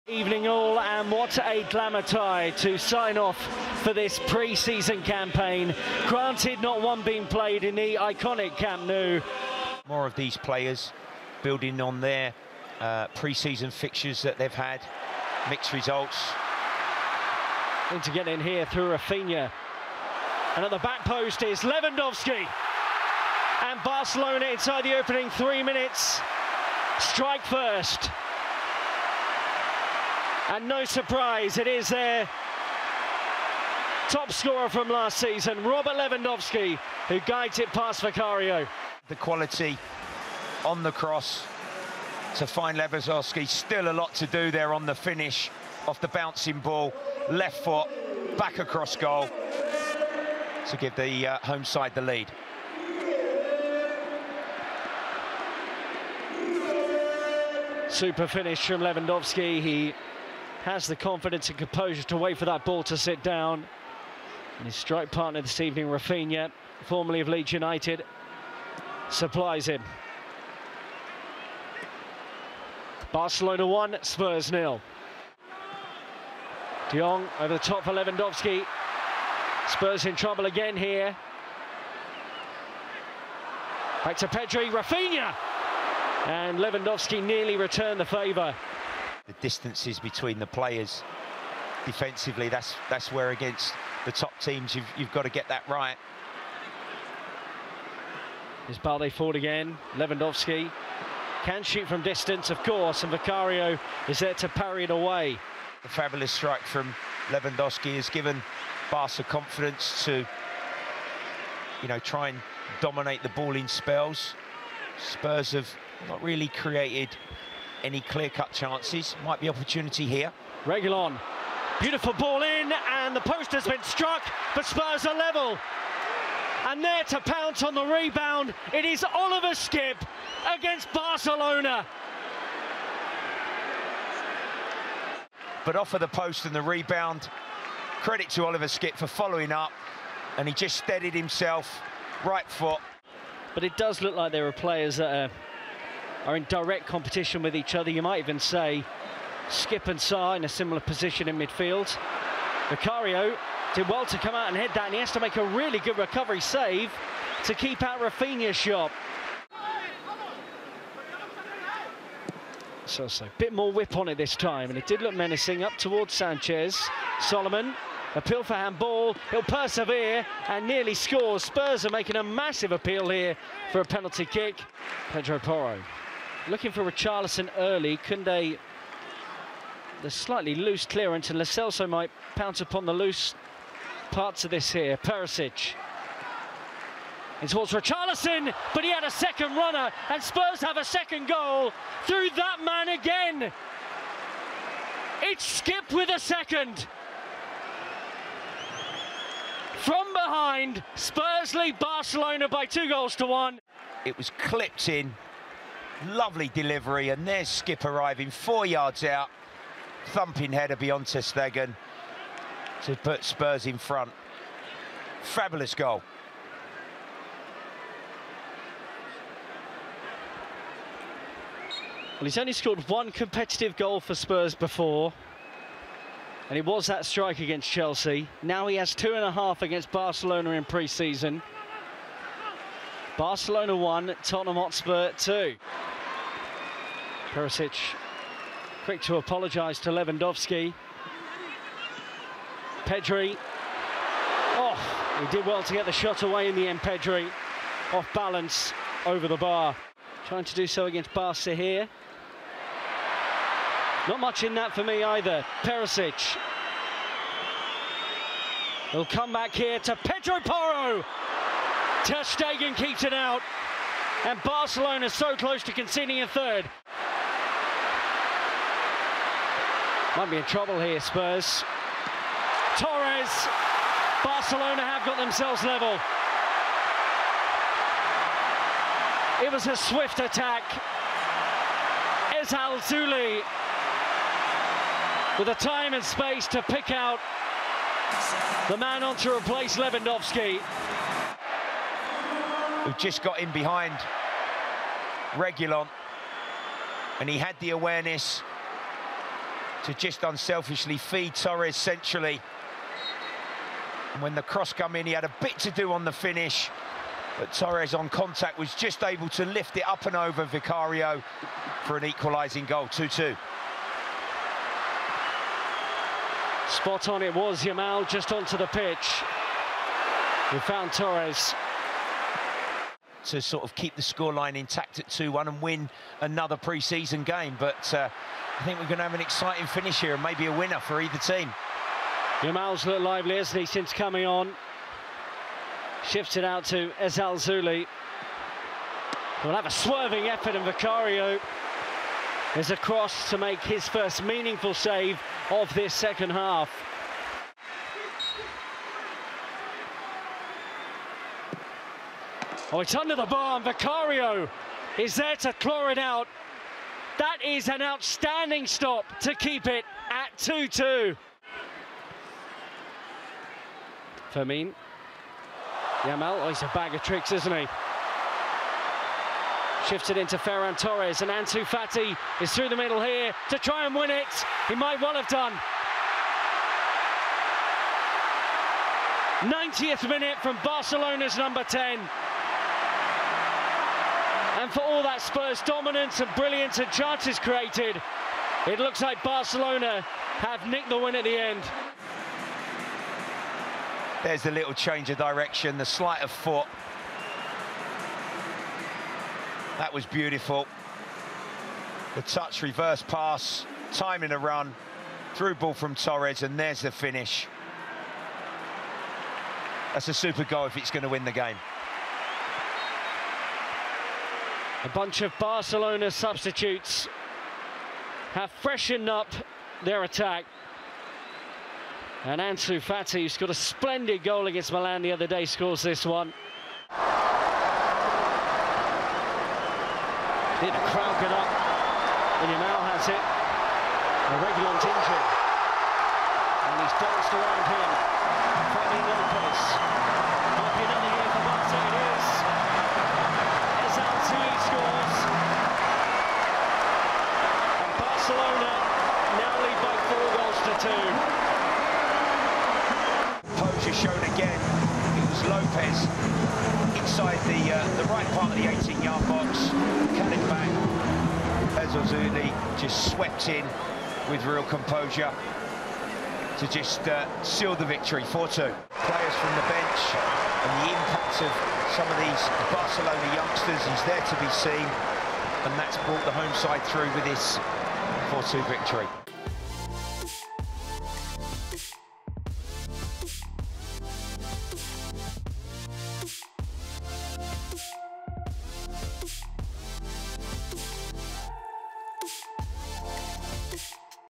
BARCELONA 4-2 TOTTENHAM HOTSPUR | HIGHLIGHTS
Watch highlights from Tottenham Hotspur's final pre-season friendly against FC Barcelona, including two goals from Oliver Skipp.